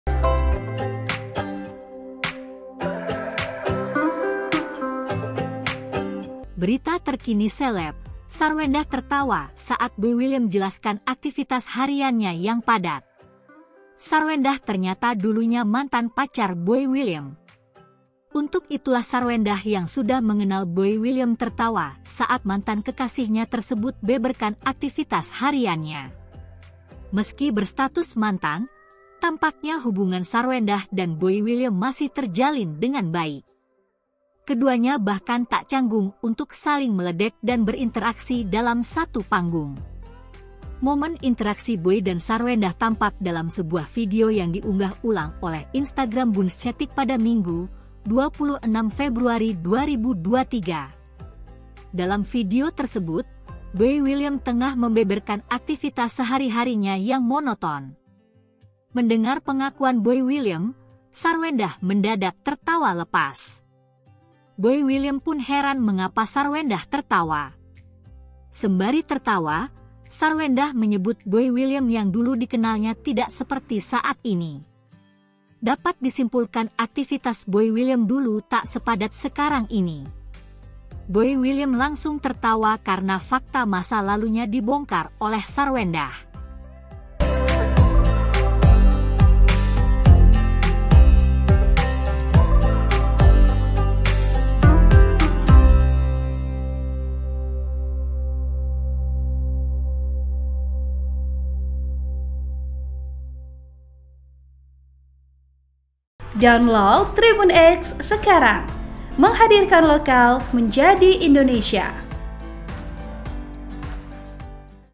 MOMEN Sarwendah Kenang Masa Lalunya dengan Boy William, Ternyata Keduanya dulu Pernah Pacaran
TRIBUN-VIDEO.COM, - Berita terkini seleb, Sarwendah tertawa saat Boy William jelaskan aktivitas hariannya yang padat.

Sarwendah ternyata dulunya mantan pacar Boy William.

Untuk itulah Sarwendah yang sudah mengenal Boy William tertawa saat mantan kekasihnya tersebut beberkan aktivitas hariannya.

Artikel ini telah tayang di TribunLampung.co.id dengan judul Sarwendah Tertawakan Boy William yang Dulu Mantannya,